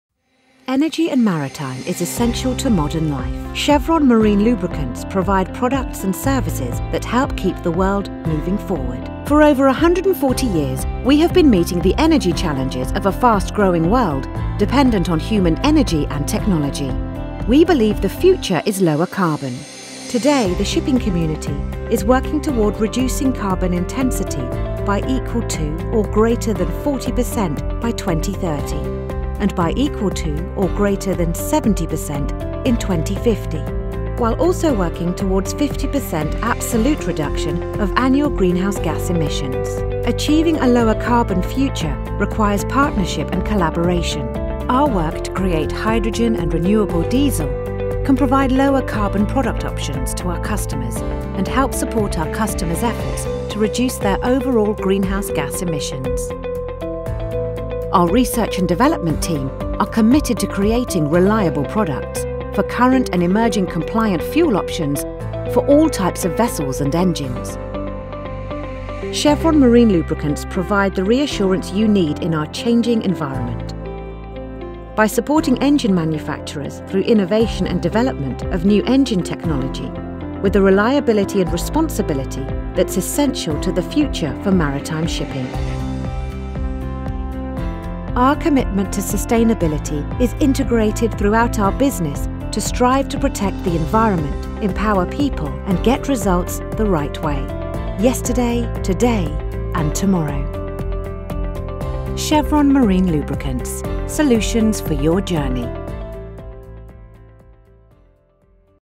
A Lower Carbon Future for Maritime
Protecting the environment while providing affordable, reliable
and ever-cleaner energy takes commitment supported by effective
processes, leading technologies, and dedicated people.
Our environmental and energy transition efforts focus on lowering the carbon intensity of our operations, building lower carbon businesses, supporting well-designed climate policy, responsible water management, and biodiversity, while using our Operational Excellence Management System to manage risk.